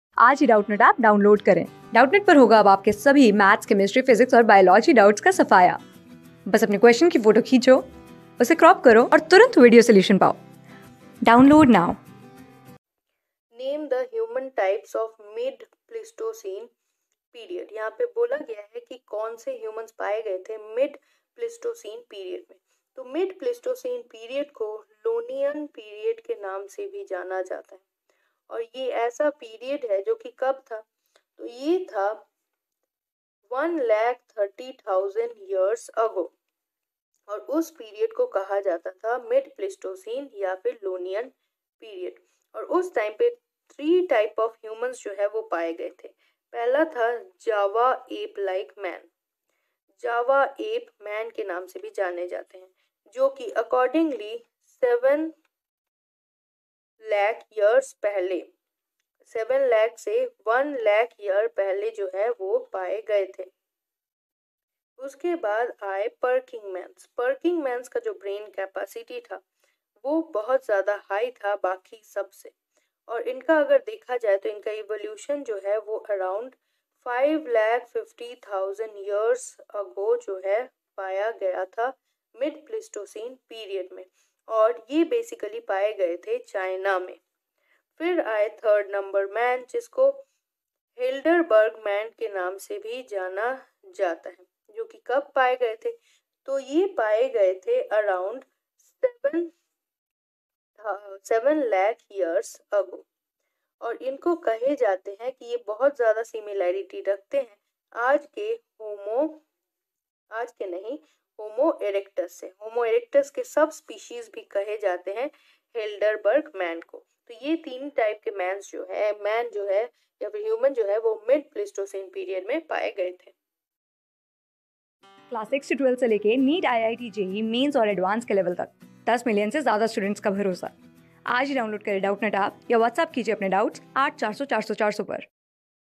Name the human types of mid-Pleistocene period. | CLASS 12 | EVOLUTION | BIOLOGY | Doubtnut
Name the human types of mid-Pleistocene period.
Class: 12
Subject: BIOLOGY
Chapter: EVOLUTION
Board:CBSE

👉 Have Any Query? Ask Us.